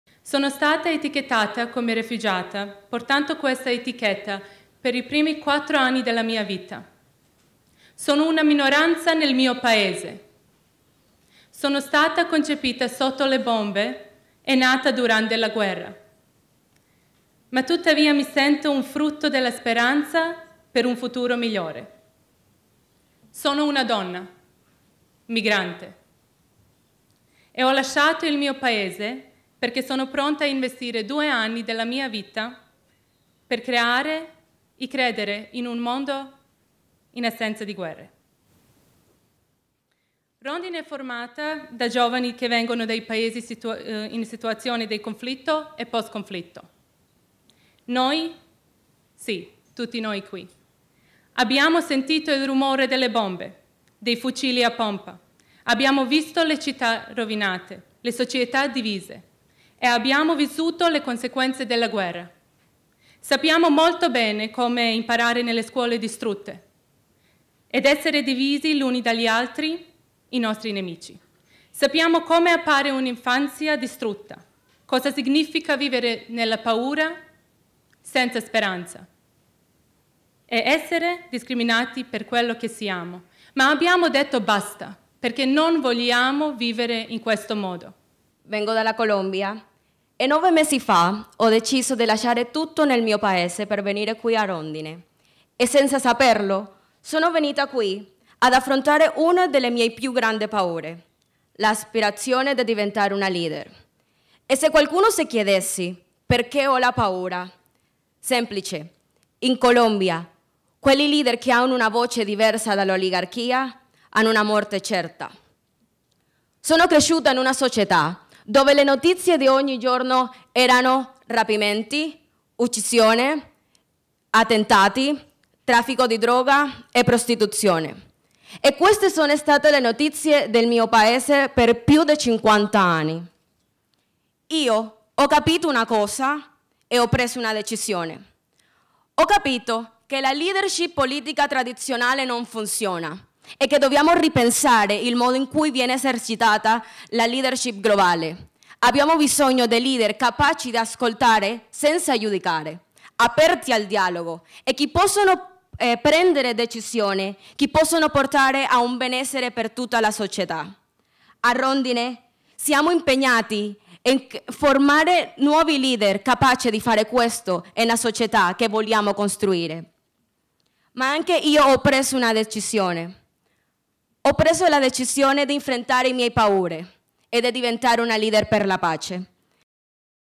Leaders for Peace: Sara e Stephanie sul palco di Rondine Cittadella della Pace
Estratto dagli interventi di Sara e Stephanie, studentesse della World House di Rondine, nel giorno in cui si conferma il sostegno dell'Italia alla campagna "Leaders for Peace" alla presenza del Presidente del Consiglio Giuseppe Conte.